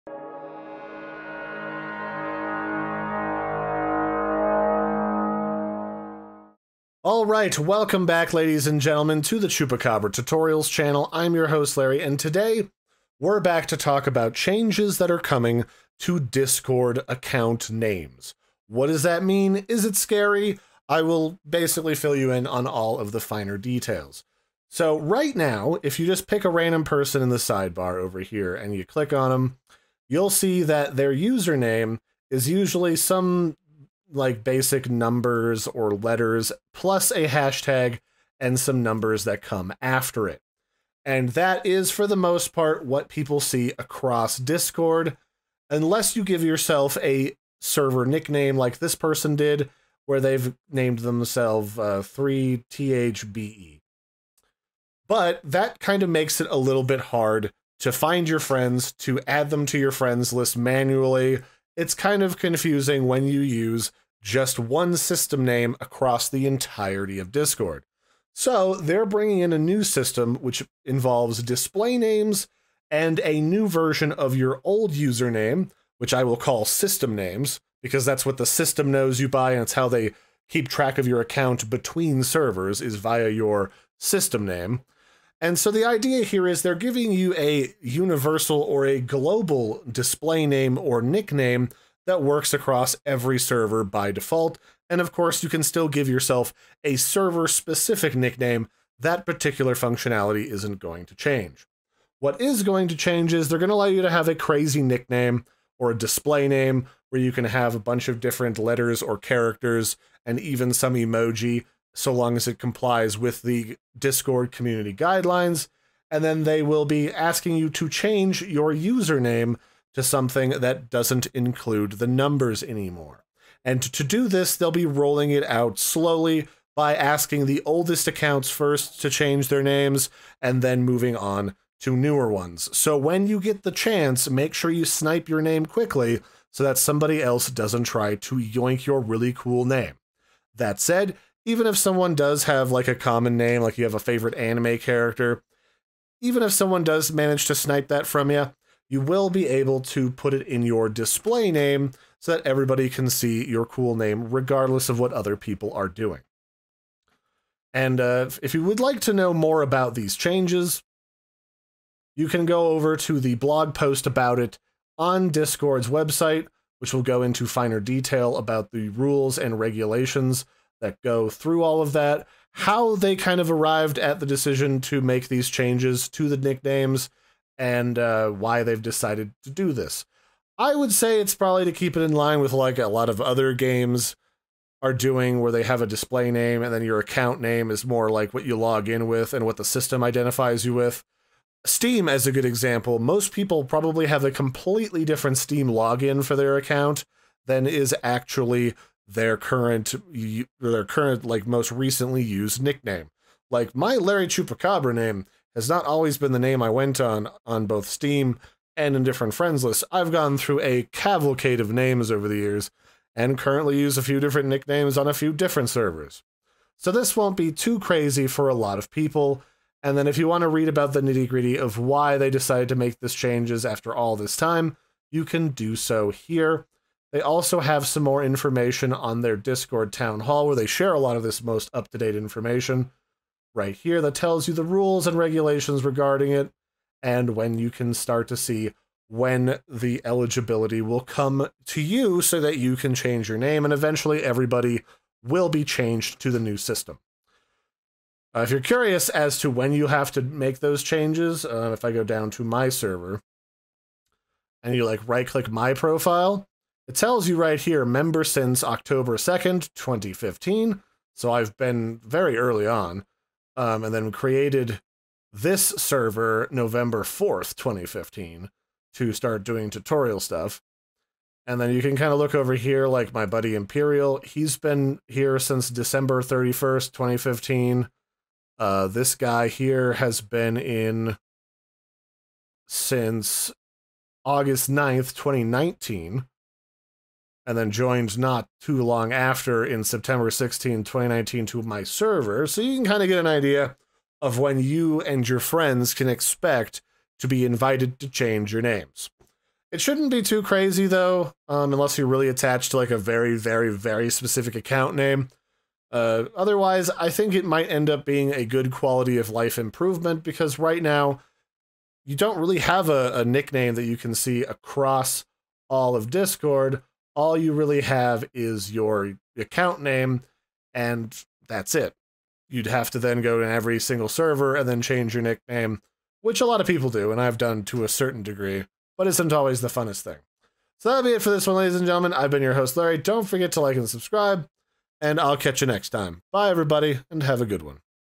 BIG CHANGES Coming to Discord UserNames & Display Names
Discord is making some pretty big changes to how they handle Usernames. Including adding a whole new Display name that acts like a nickname across the entire platform. So now you'll have both a username used by the system to keep track of your account, and a Display name that easier to remember so your friends can easily find you, add you to their friends list, and more!